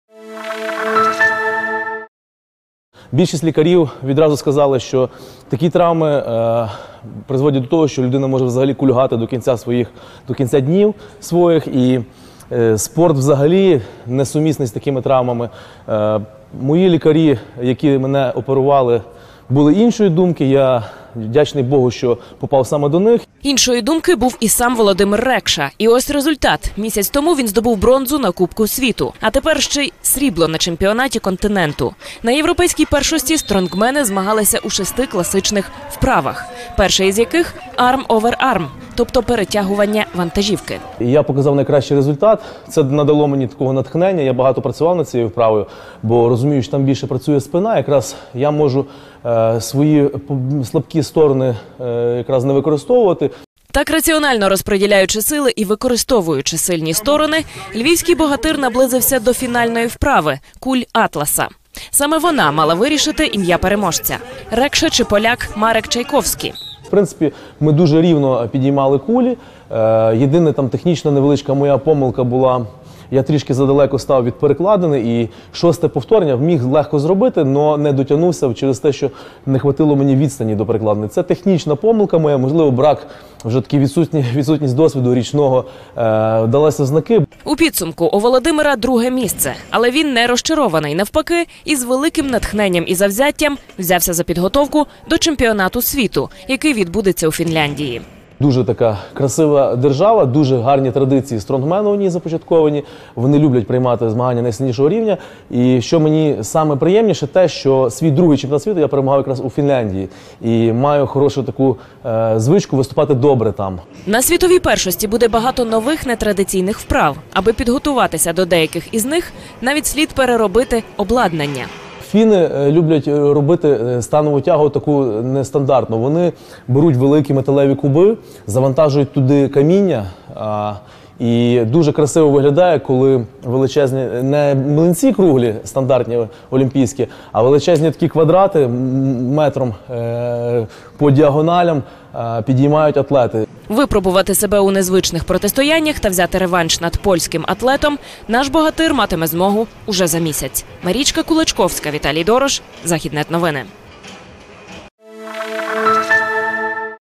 Львівський стронгмен Володимир Рекша здобув «срібло» на чемпіонаті Європи
Львівський стронгмен Володимир Рекша повертається на міжнародну арену після важкої травми. Минулоріч на змаганнях силач так пошкодив ногу, що опинився під загрозою завершити кар`єру. Розповідає: «Більшість медиків казали, що такі травми призводять до того, що людина може кульгати до кінця своїх днів. І спорт взагалі не сумісний з такими пошкодженнями. Але лікарі, які мене оперували, були іншої думки. Я вдячний Богу ,що потрапив саме до них».